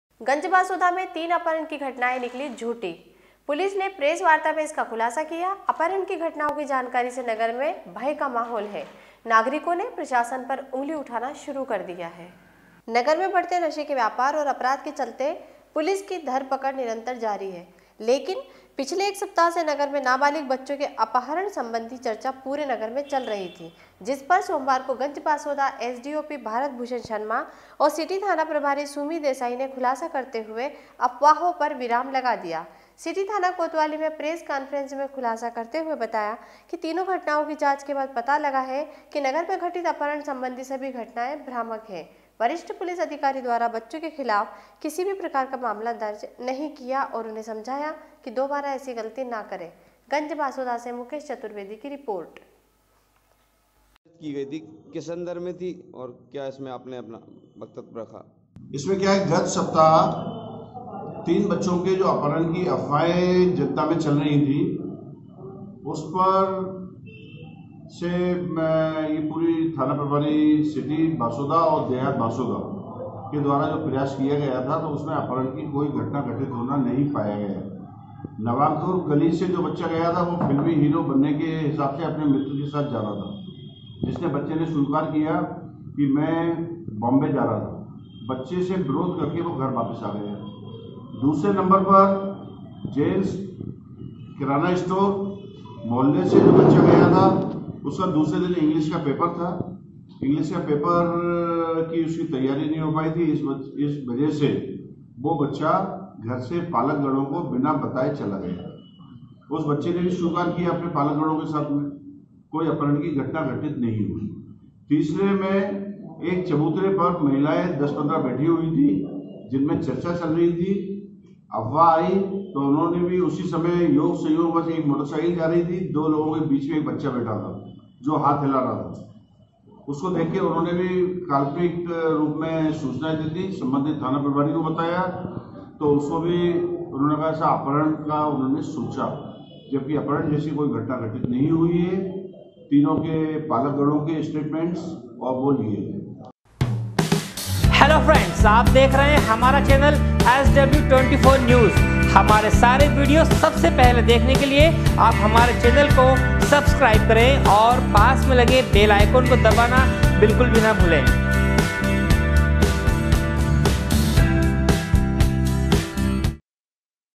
Ganjbasoda- गंजबासौदा में तीन अपहरण की घटनाएं निकली झूठी। पुलिस ने प्रेस वार्ता में इसका खुलासा किया
Ganjbasoda- SW 24 News Madhyapradesh
गंजबासौदा में तीन अपहरण की घटनाएं निकली झूठी। पुलिस ने प्रेस वार्ता में इसका खुलासा किया है। अपहरण की घटनाओं की जानकारी से नगर में भय का माहौल है। नागरिकों ने प्रशासन पर उंगली उठाना शुरू कर दिया। गंजबासौदा से मुकेश चतुर्वेदी की रिपोर्ट